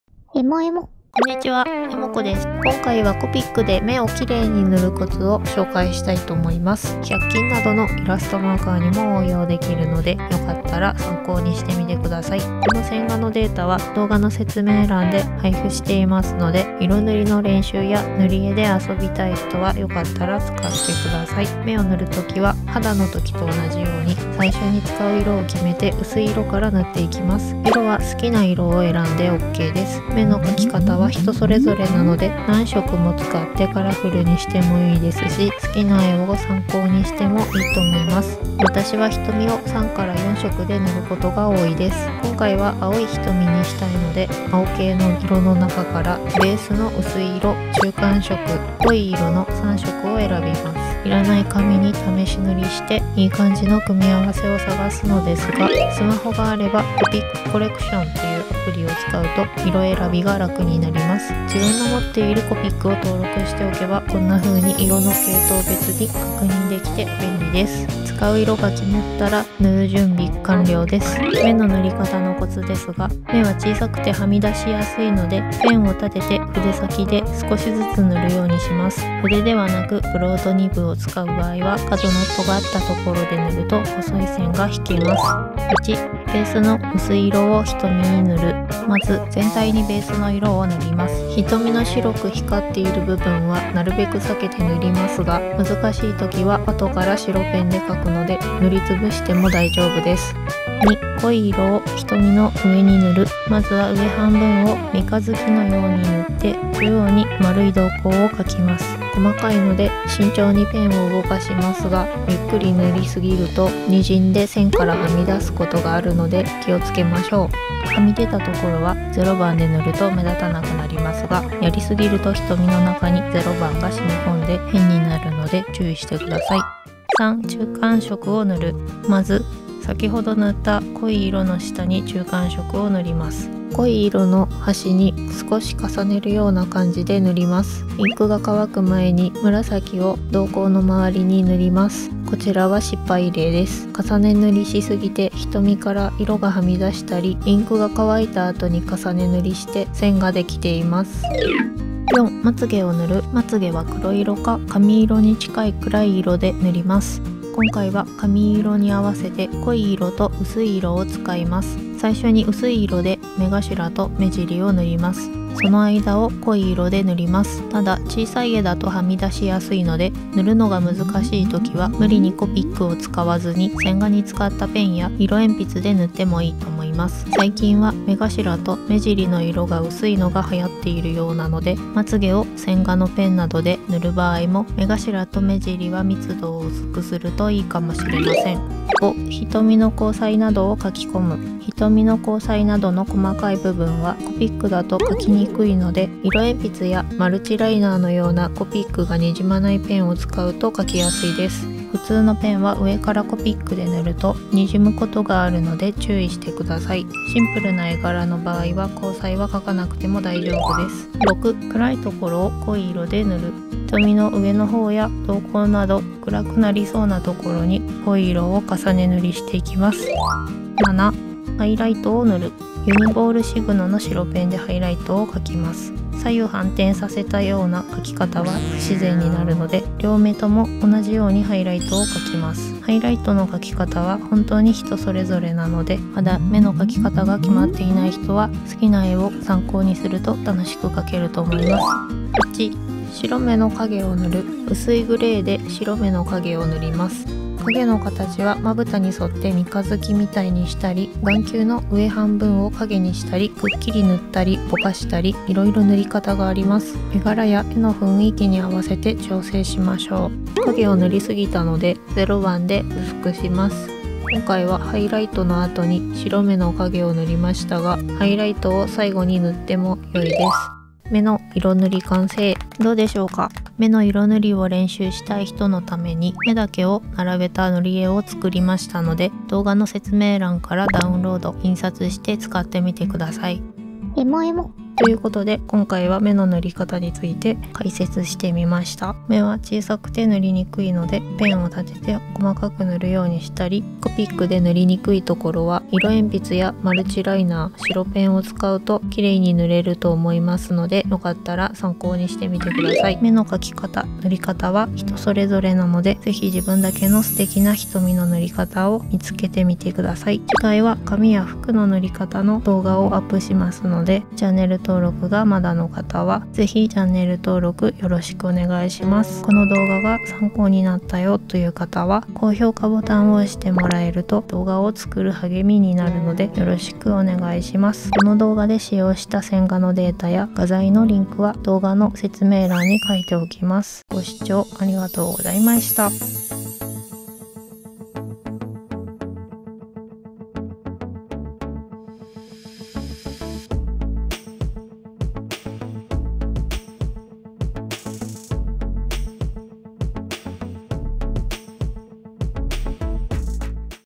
【コピック・百均】目の塗り方講座🖊✨失敗しないコツを解説します❗️【初心者向け】How to paint eyes with copic
コピック初心者さん向けに、目の塗り方を解説しました！（百均マーカーにも応用できます）
⬇️ お絵かき講座リスト ⬇️

★ 作業環境 ★
⬇️ コピックチャオ ⬇️

⬇️ uniボールシグノ ACホワイト ⬇️

⬇️ CLIP STUDIO PAINT PRO ⬇️

⬇️ Wacom Cintiq 16（液タブ）⬇️

⬇️ iPad Pro 2020年モデル ⬇️

⬇️ Apple Pencil 第2世代 ⬇️